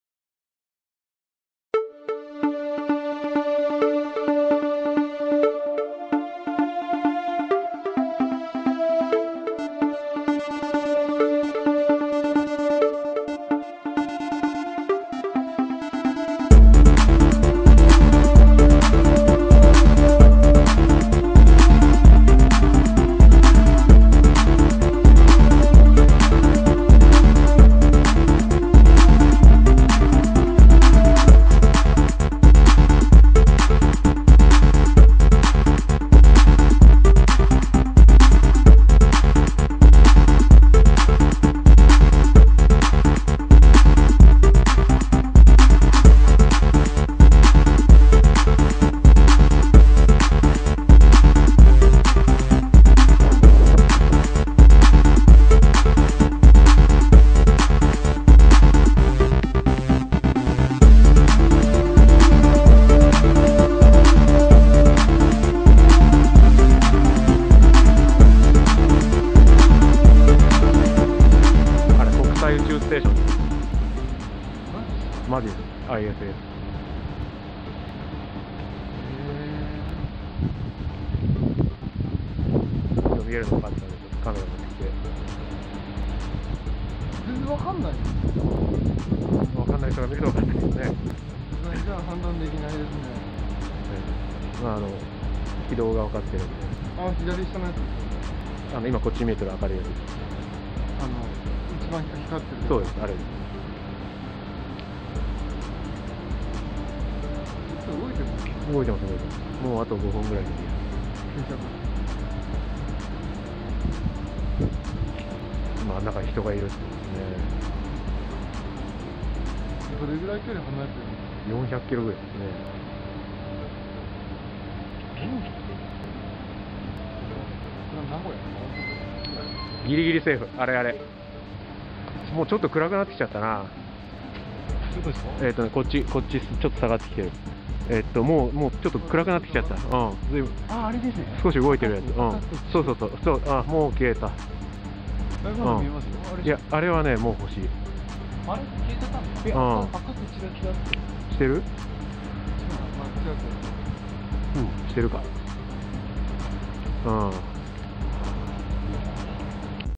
ISS (International Space Station) , Venus and Jupiter
ISS (International Space Station) passes by Venus and Jupiter on Nov. 21, 2008. Viewed from workplace building roof in Odawara. ISS（国際宇宙ステーション）が金星、木星の横を抜けてゆくところをデジカメで撮影。会社の屋上で撮っていると、タバコを吸いに来た知らない人に怪しまれ、なんだか間抜けな会話とあいなりました。